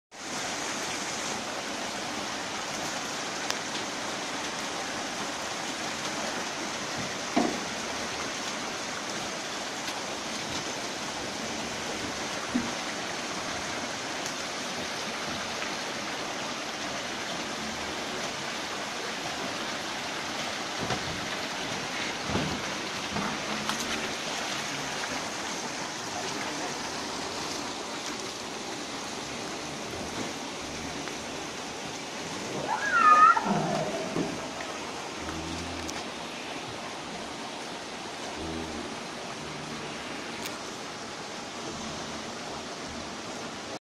Menurunkan Sapi dari Mobil Pengangkut Sapi
Menurunkan sapi potong ke kandang penggemukan sapi potong. Salah satu kegiatan anak kandang adalah mengambil sapi dari pasar sapi atau dari peternakan ke kandang untuk digemukkan kembali dan siap untuk di potong sebagai sapi Pedaging atau sapi yang di manfaatkan dagingnya untuk si konsumsi